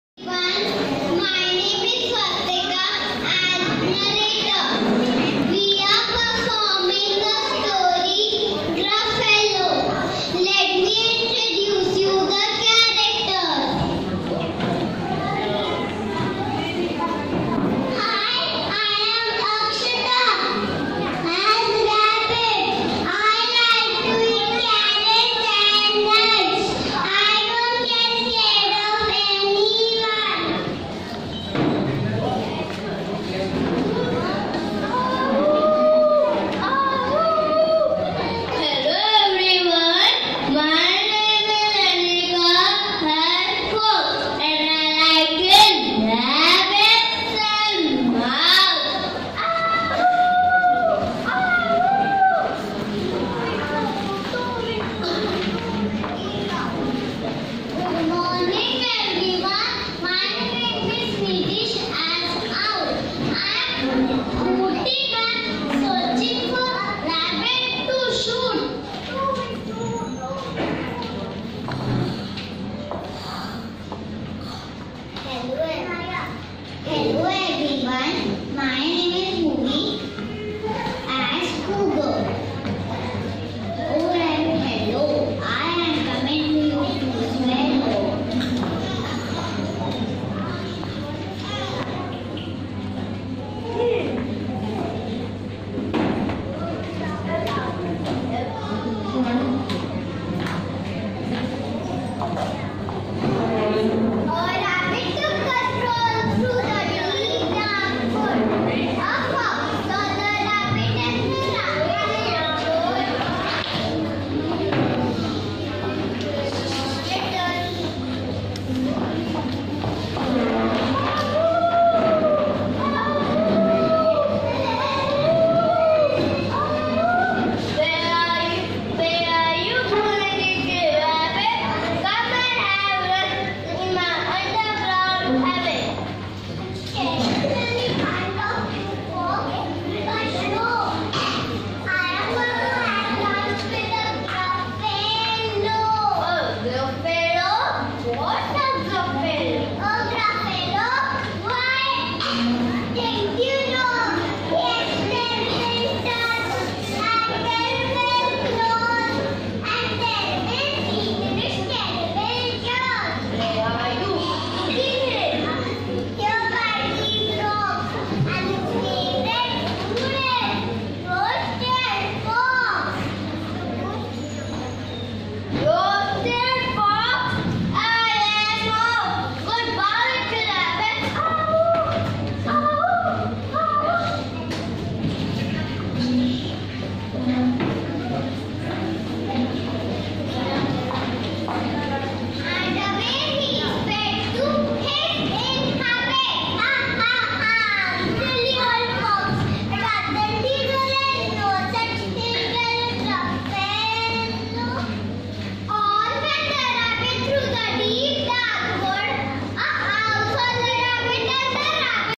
GRUFFALO (Part 1)
Gruffalo performed by 5yr old kids of Highbrow Junior school for Trinity SPEECH N DRAMA  Exams